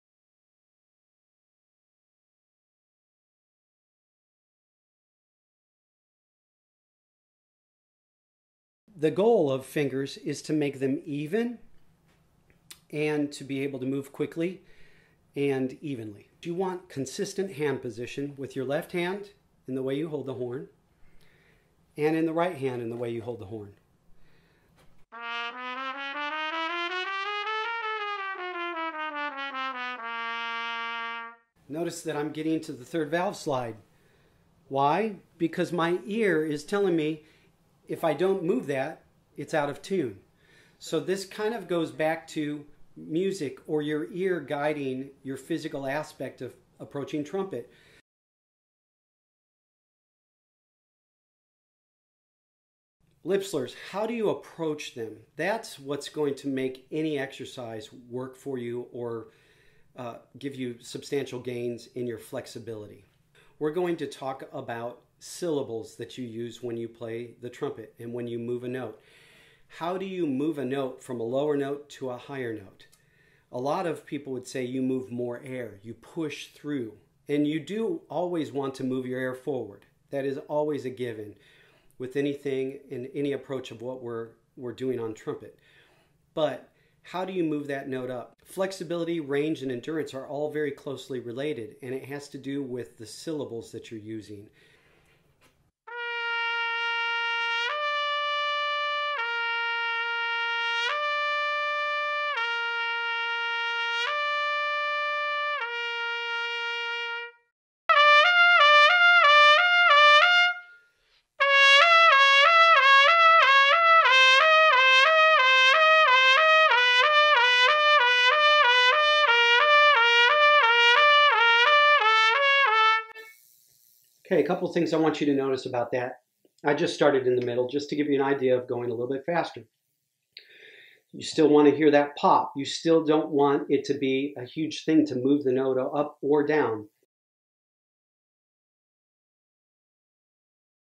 Steve Patrick Trumpet Fundamentals Fingers and Flexibility Lessons/W Studio Musicians
Preview of episodes 7 and 8 in trumpet fundamentals series. Fingers and flexibility.